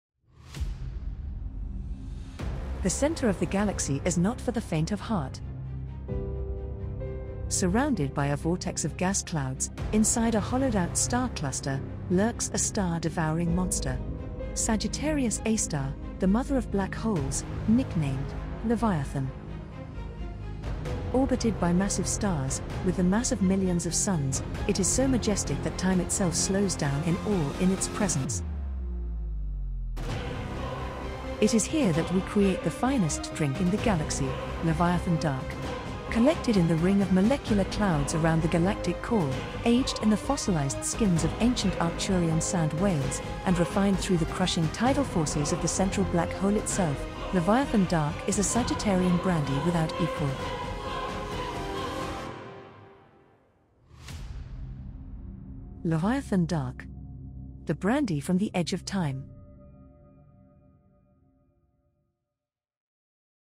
Alien Ad: From the centre of the Galaxy...
Sagittarius A* is the massive black hole in the centre of the Milky Way. It is here that we produce the finest Sagittarian Brandy in the Galaxy: Leviathan Dark.

Ad will appear in the episode "What is time".